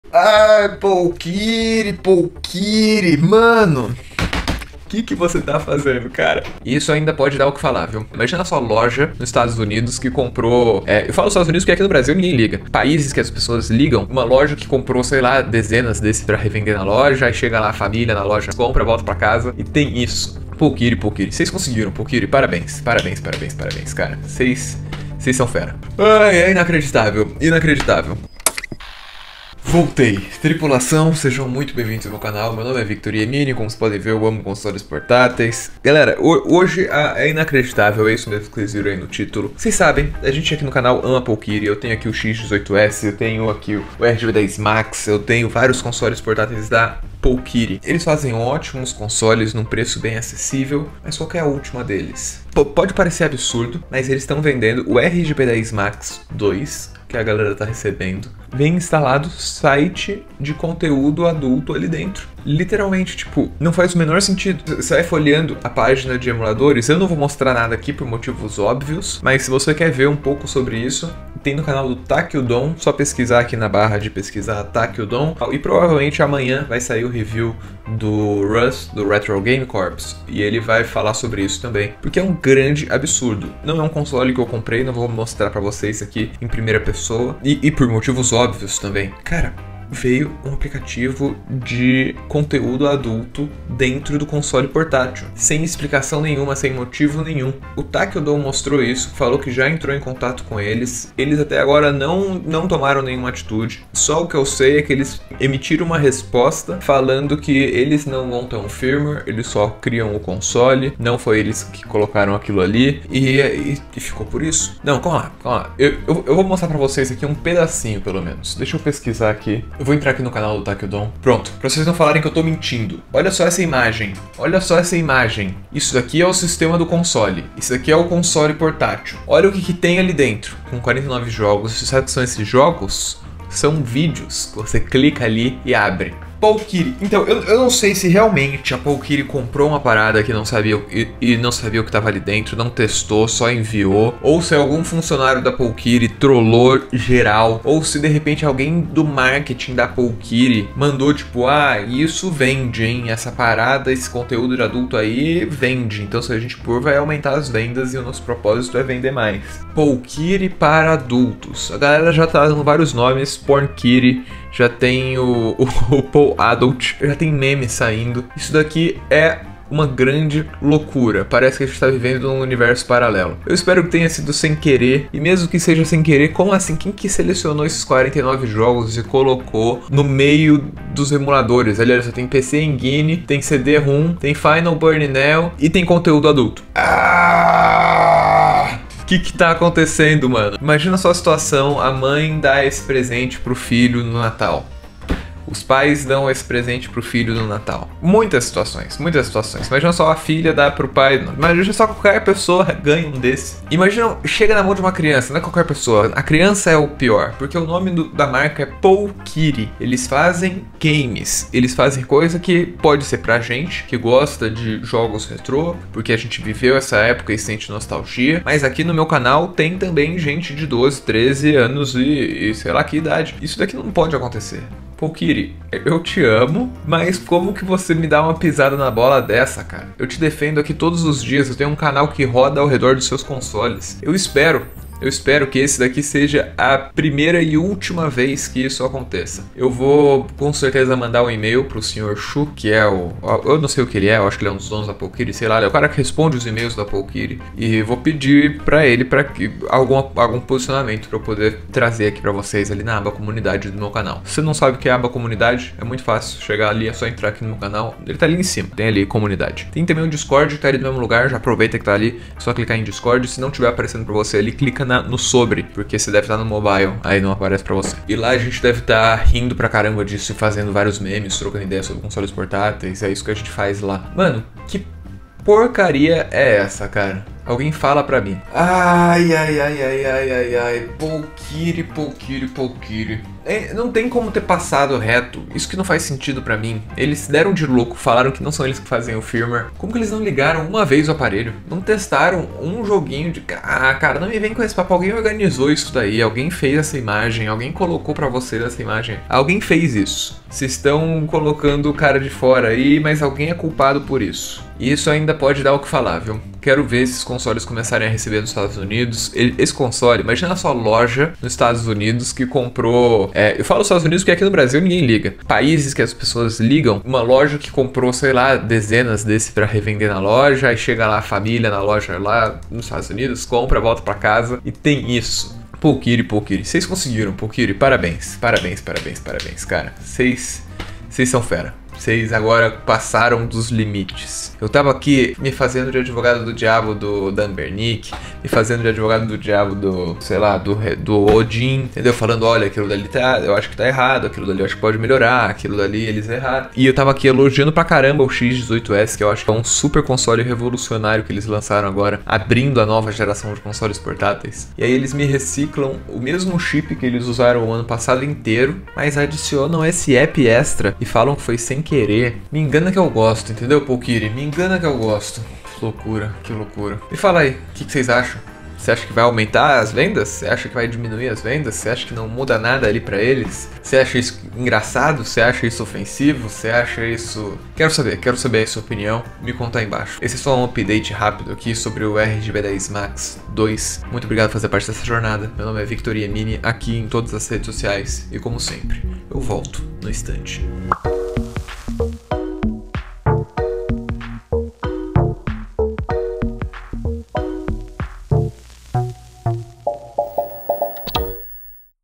POWKIDDY PARA ADULTOS! não compre esse console!
Dessa vez a marca de consoles portáteis PowKiddy pisou na bola com seu último lançamento,  o RGB10MAX 2 já é conhecido por ser o primeiro console portátil com um aplicativo impróprio para menores de idade. Fiquem atentos e não compre esse console para crianças! COMPARTILHE!